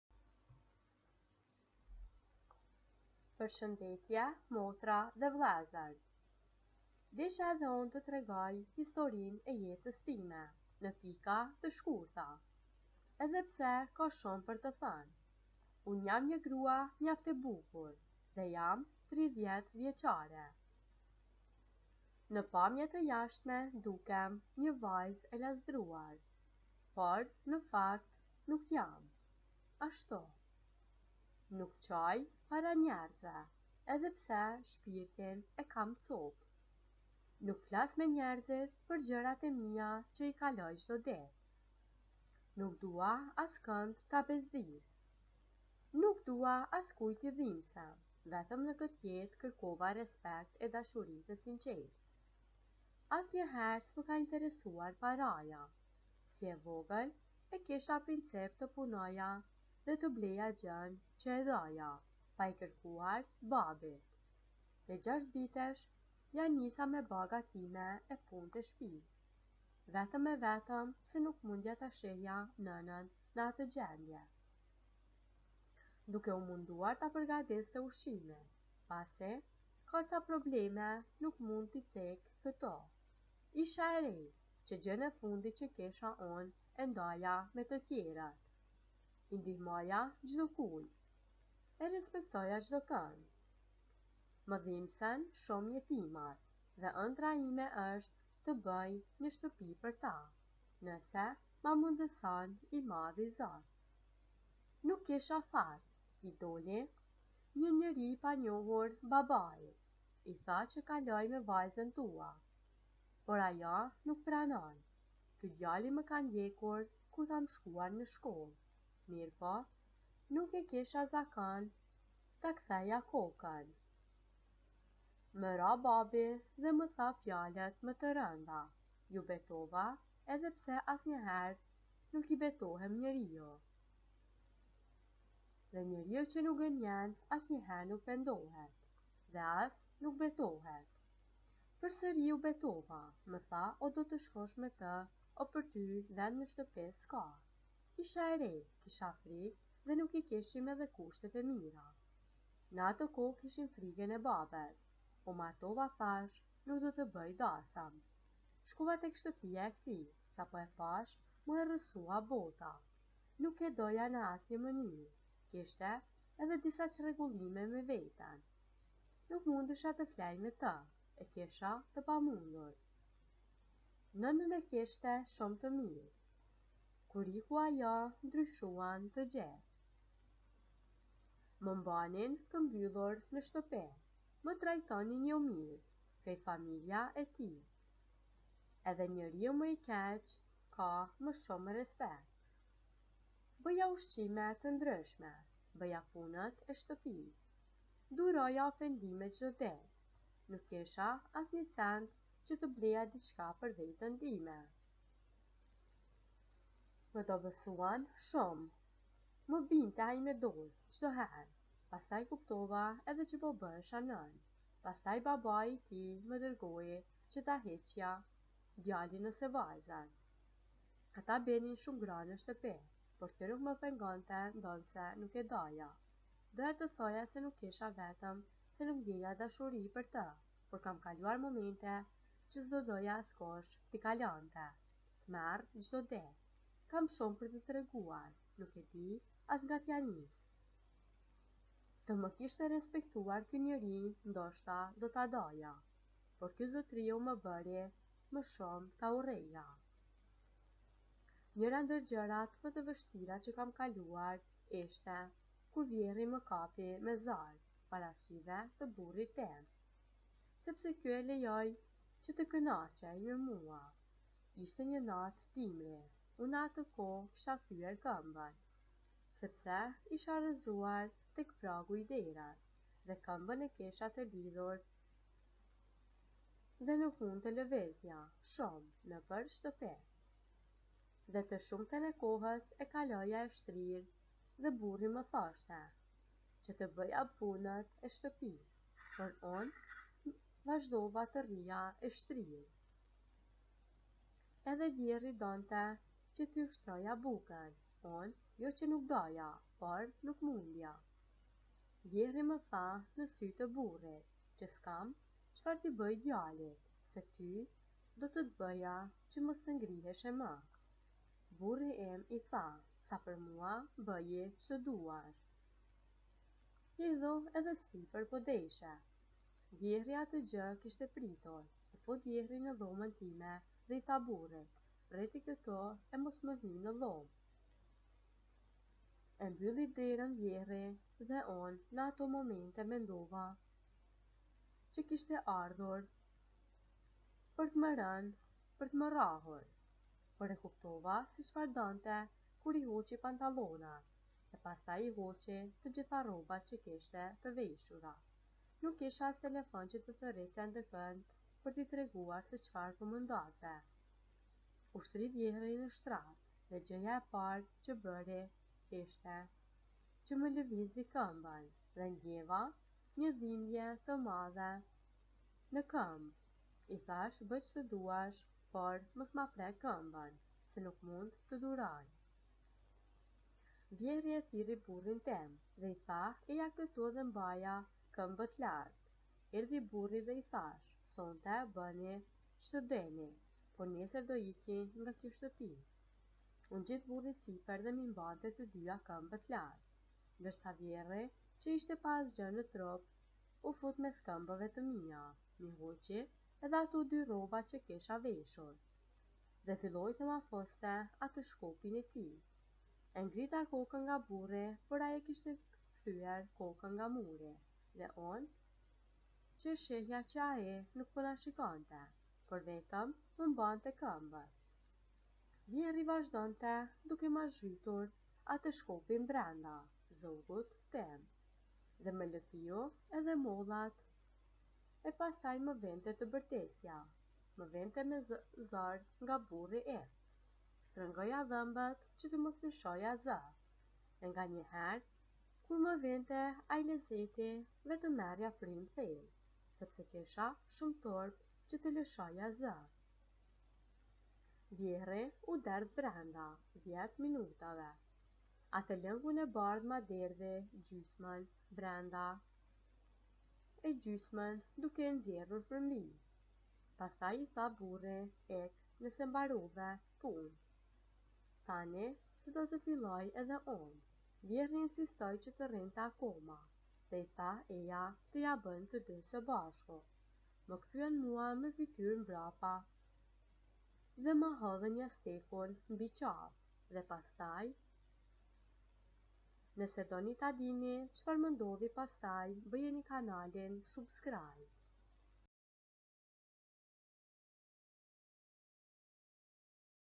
Shetitja ne Bore
Ne Fshat ra Bore dhe ne shkuam te shetisim